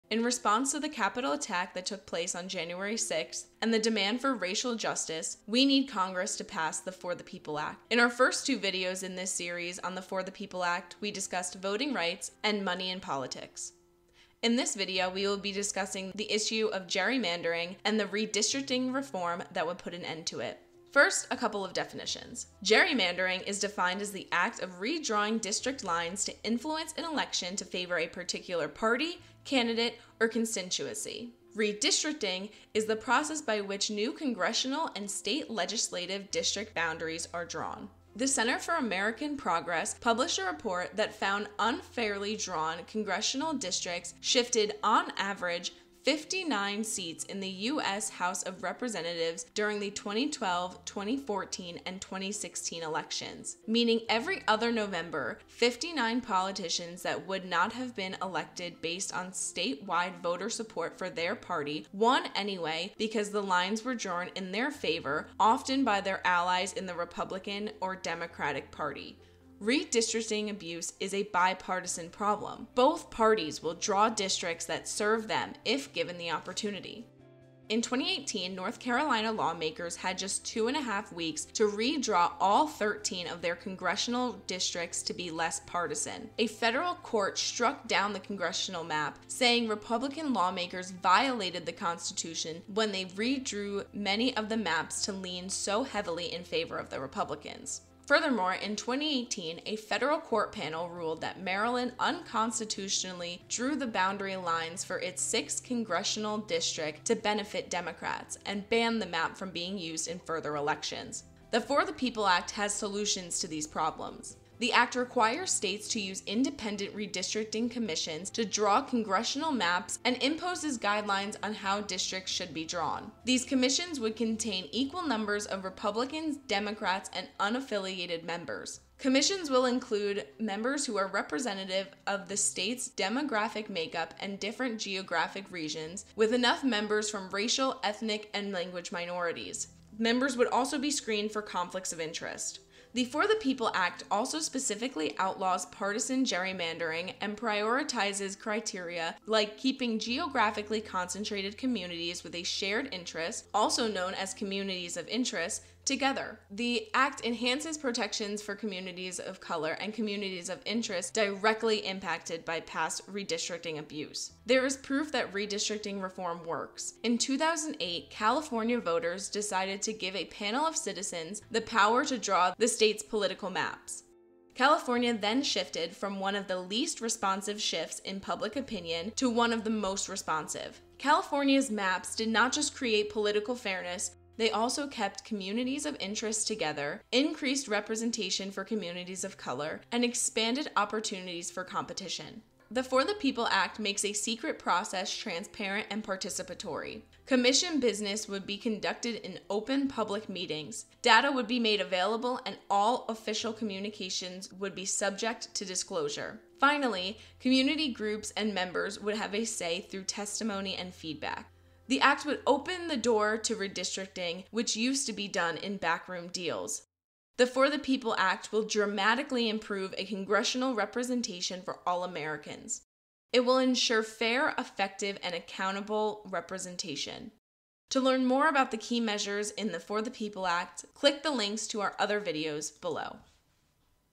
For the People Act Explained: Redistricting Reform
The For the People Act will modernize American politics, including the way we vote, the role of money in politics, and how political maps are drawn.